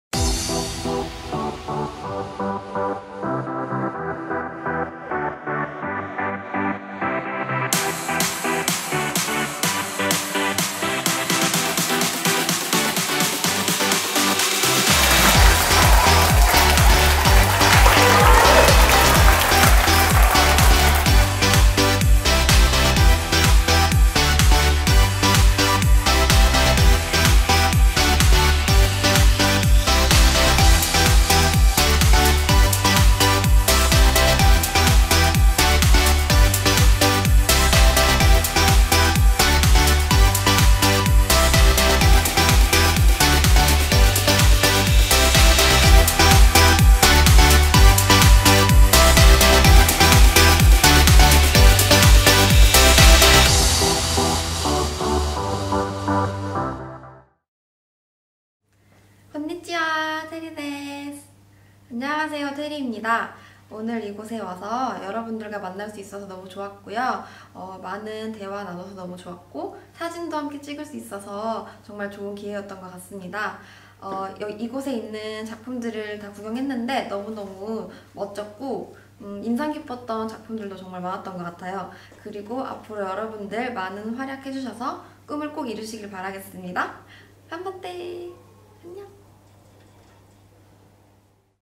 バンタンデザイン研究所 | 『カンテリさん（Taeriちゃん）来校イベント』 アフタームービー
ファッション・ヘアメイク(韓流メイク含む)・美容師・グラフィックデザイン・映画・映像・写真等のクリエイティブ総合専門校のバンタンデザイン研究所(Vantan Design Institute)にInstagramを中心に大人気の韓国人モデル&インフルエンサーの「カンテリ」さんが来校！ファッションや美容についてトークショーを実施しました。カンテリさんをモデルにしたメイクの実演や、フォト（写真）シューティングや、ヘアメイク・スタイリストのトークセッションなどバンタンならではのスペシャルなイベントに。動画のラストには参加者に向けたスペシャルメッセージも収録！